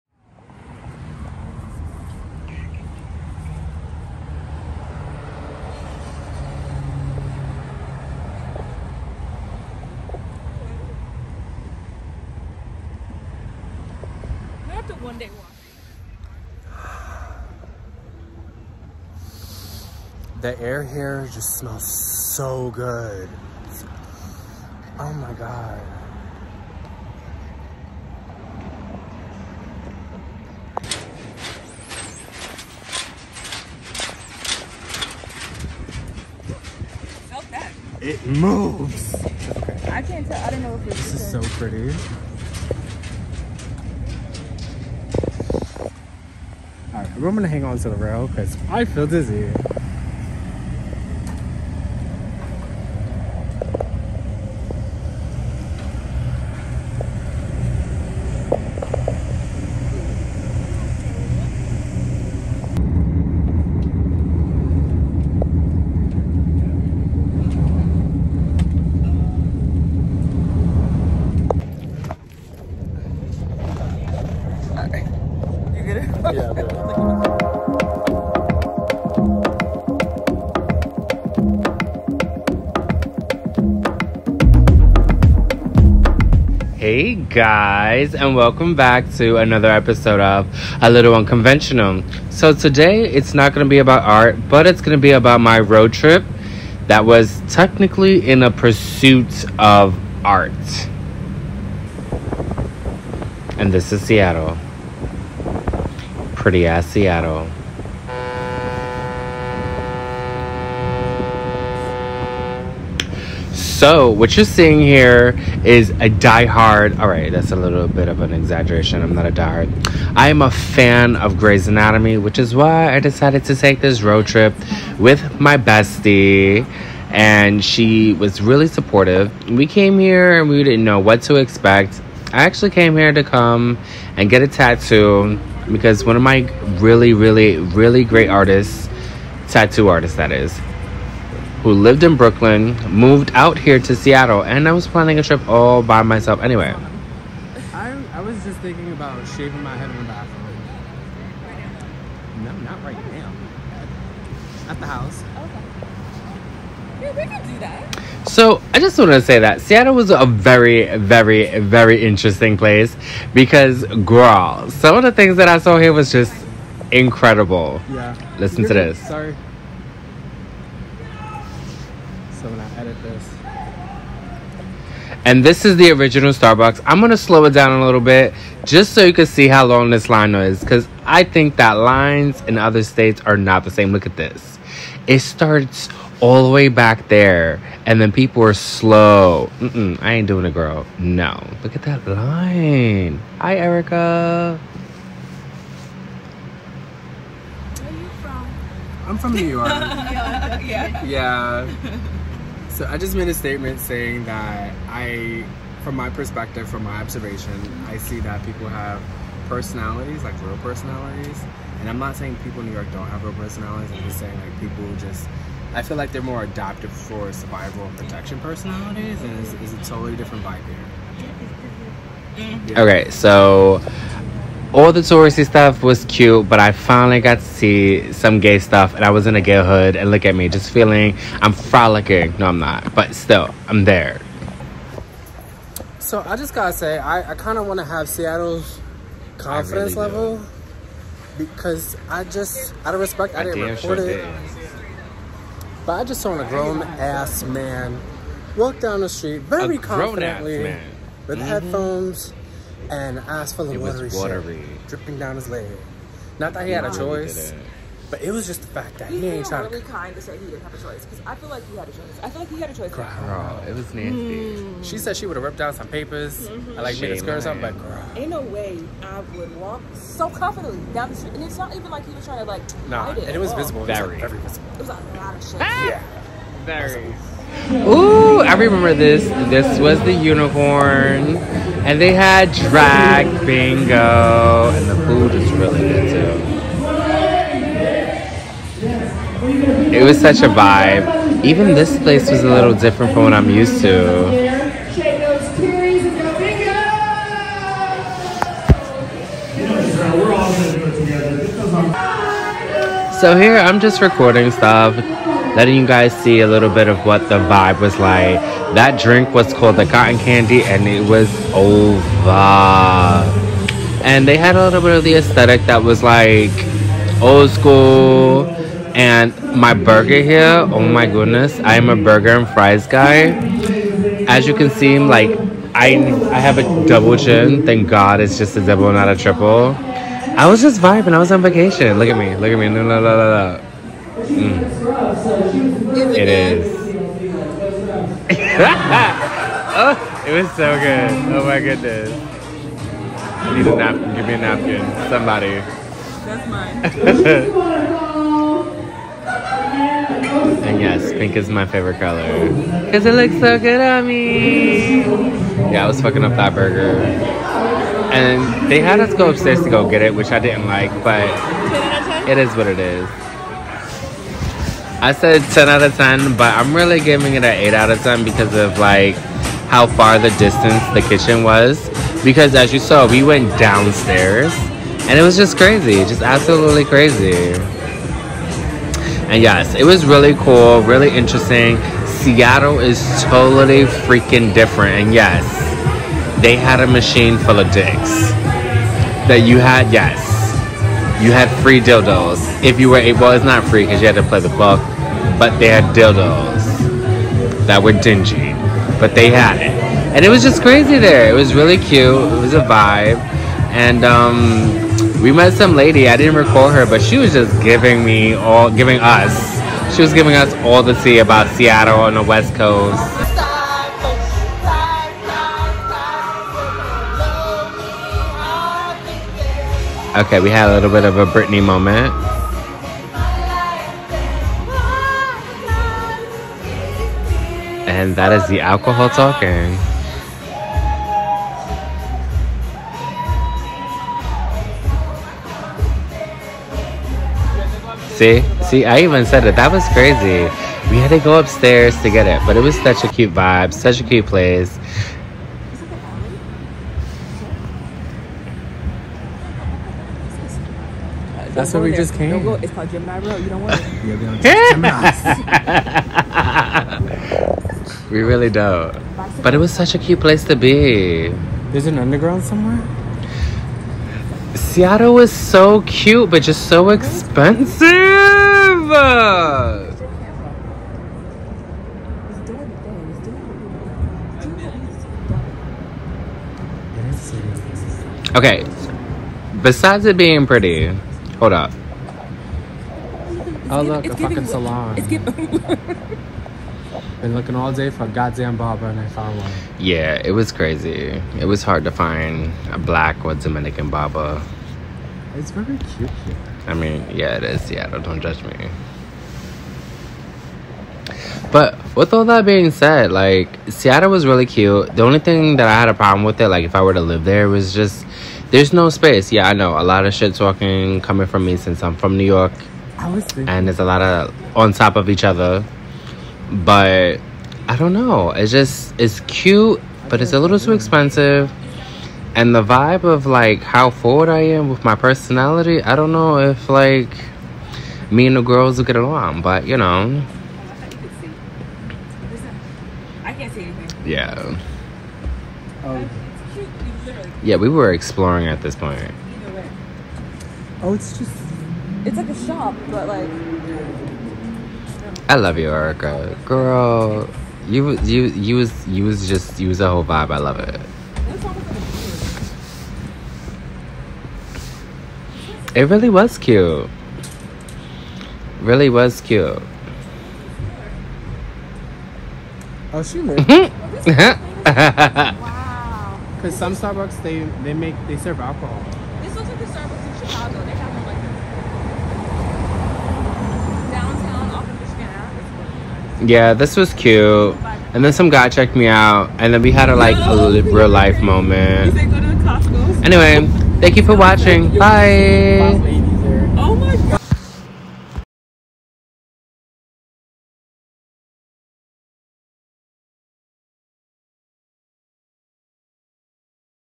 Seattle RoadTrip 😁
Here, my best friend Erica and I. Went to seattle washington to meet up with my tattoo artist. It did not happen because of poor planning. But I have to say Seattle was very intresting and had gothamcity vibes at night with all of its homeless population. I filmed this on an iphone12promax and a sonyzv1 we were able to see the 1st starbucks it was cute. But I was not waiting on that line. However being there I have learned so much about myself. bestfriends roadtrip #2023